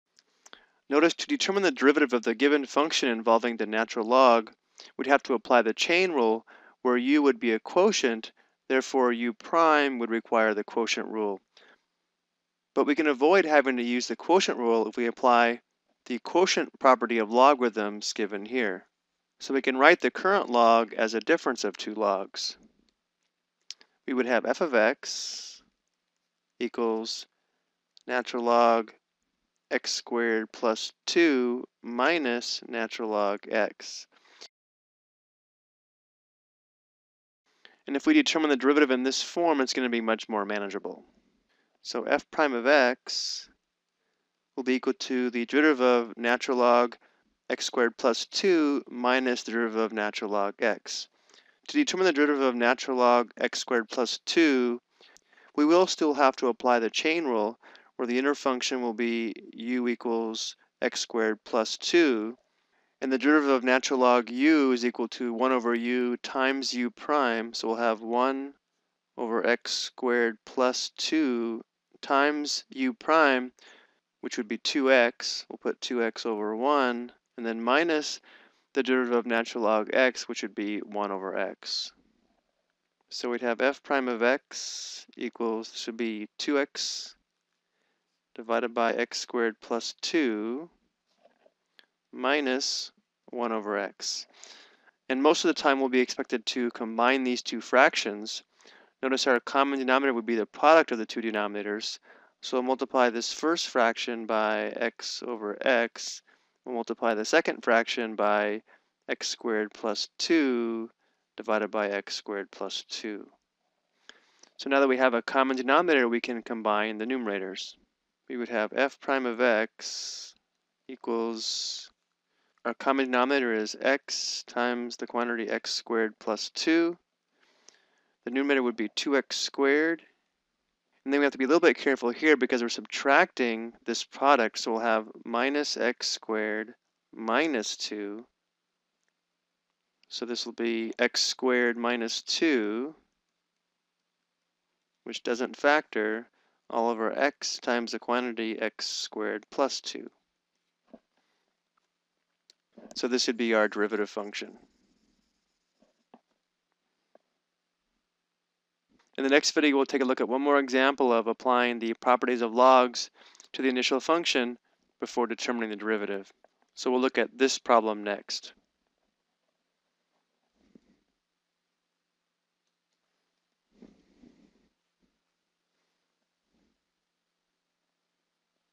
Ex 7: Derivatives of the Natural Log Function using Log Properties
This video provides an example of determine the derivative of a natural log function by applying the properties of logs before determining the derivative.